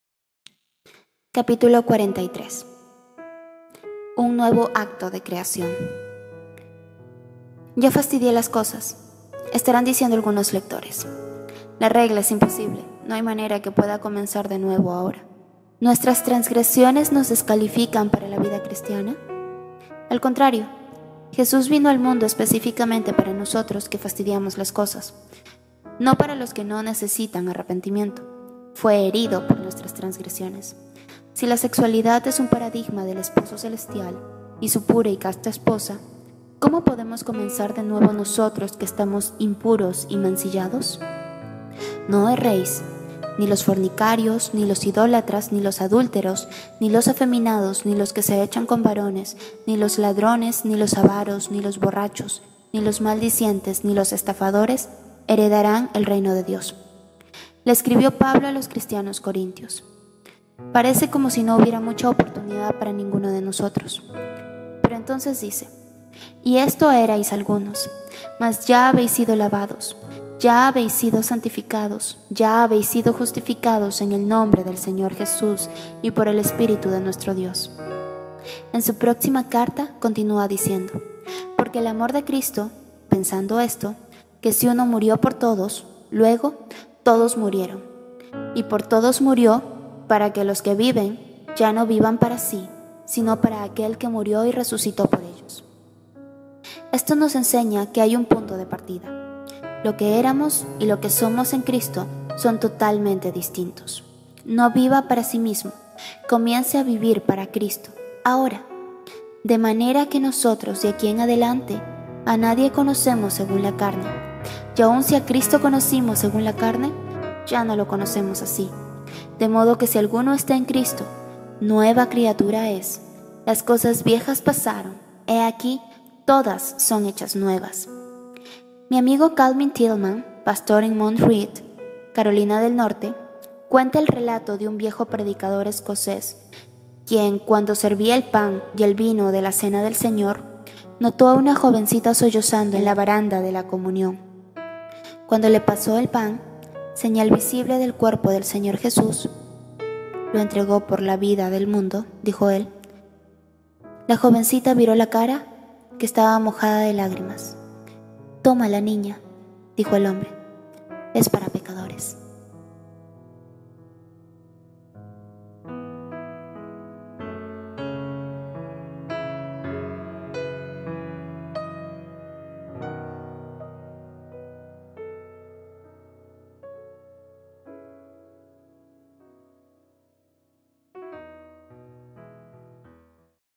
UN NUEVO ACTO DE CREACIÓN - Final | Elizabeth Elliot: Pasión y Pureza - Cap.43 (Audiolibro)
"Este es un libro sobre cómo someter nuestra vida amorosa a la autoridad de Jesucristo. Elisabeth lo ha hecho cálidamente personal, y respalda sus argumentos con recuerdos, diarios y viejas cartas de amor". — Ruth Bell Graham

 ¿Cómo se lidia con la impaciencia de estar con la persona que uno ama?
¿Cuál es el papel de la mujer soltera? ¿Esperar?
¿Cómo sabe uno si esa persona es la que nos conviene?
Estas y muchas otras preguntas se las han hecho a Elisabeth Elliot en respuesta a sus libros y conferencias. En Pureza y pasión la autora enfatiza la necesidad de entregar a Cristo todas las cuestiones del corazón y esperar en Él. Enseña esta a menudo dolorosa pero remunerativa disciplina relatando con candidez su historia de amor con Jim Elliot, para que "sirva de evidencia de que he pasado por eso" A través de cartas, anotaciones en su diario y recuerdos, nos habla de las tentaciones, dificultades, victorias y sacrificios de dos jóvenes cuya consagración a Cristo estaba por encima del amor que se tenían. Estos reveladores detalles personales, combinados con una enseñanza bíblica relevante, pudieran ayudarnos a recordar que sólo pasando nuestras pasiones humanas por Su fuego puede Dios purificar nuestro amor.

Autor: Elizabeth Elliot
Editor: Thomas Nelson Inc, 1995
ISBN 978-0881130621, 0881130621
Nº de páginas: 206 páginas